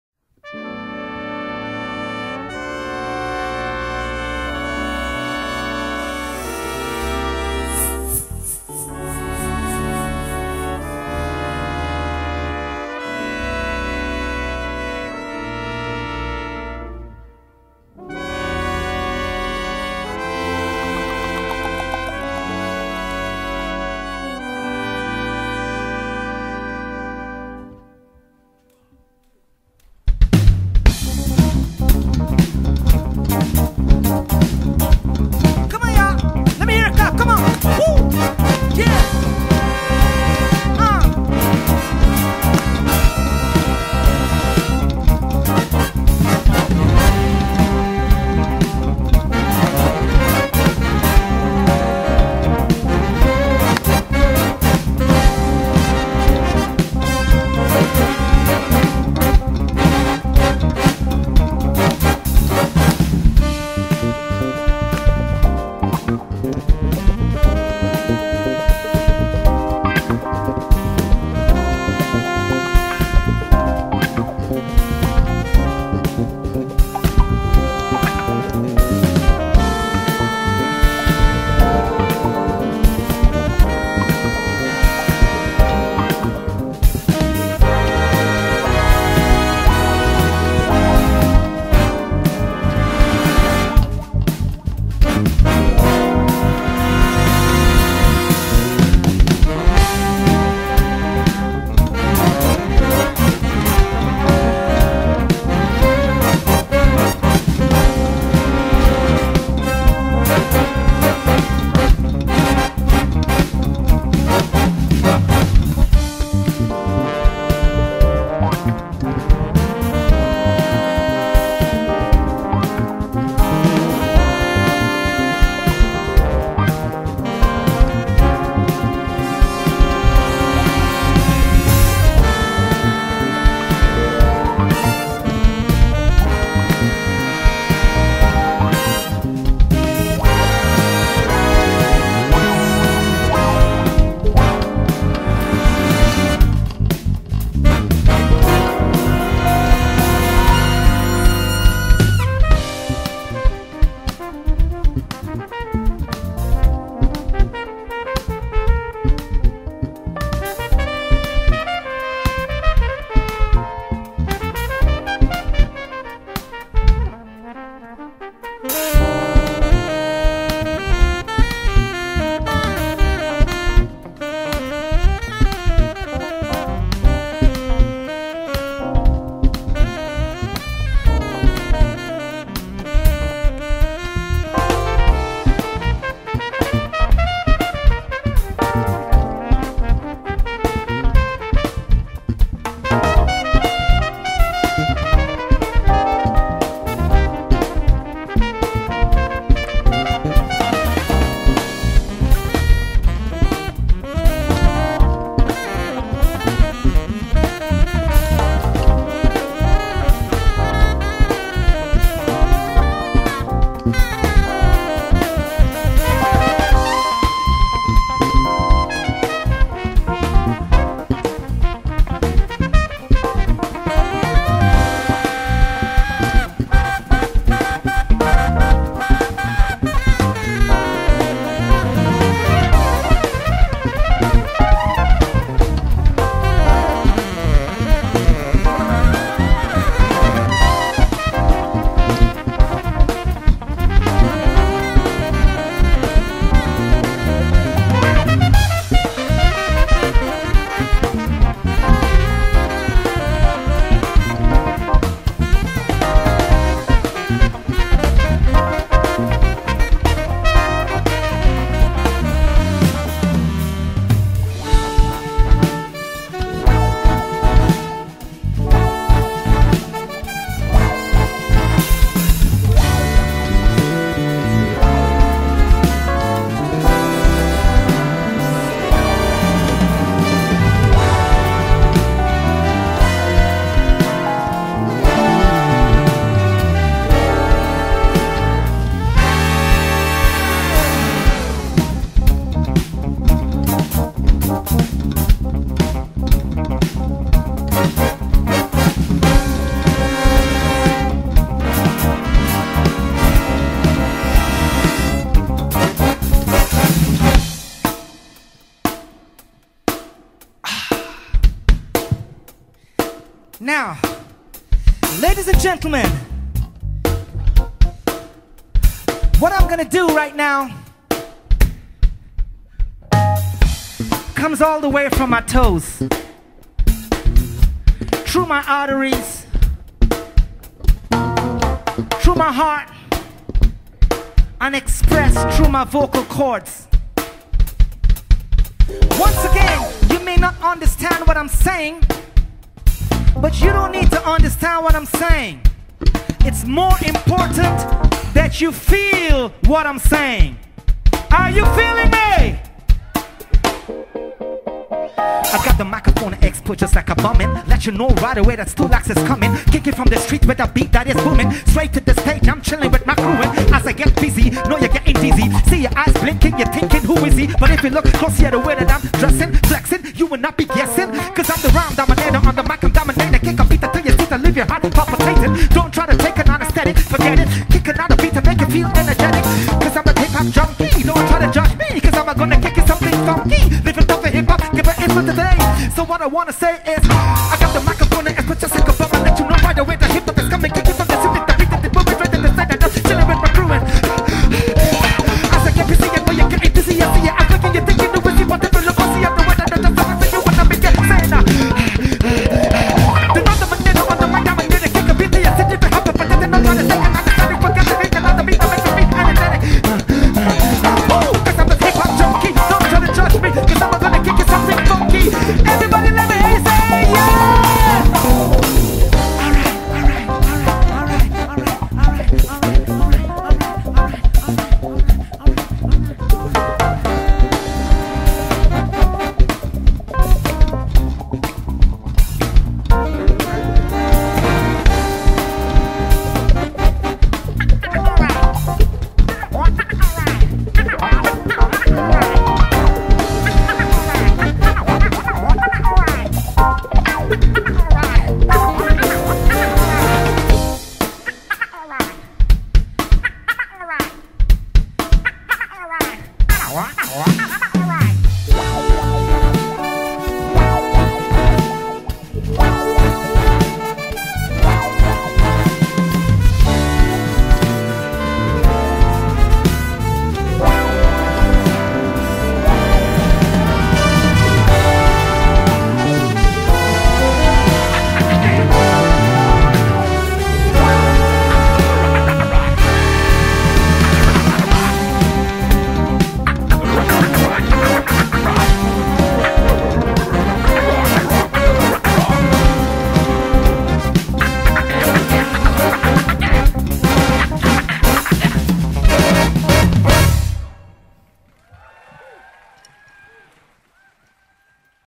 Big Band Małopolski
Big Band Małopolski live at Młyn Jazz Festival 2016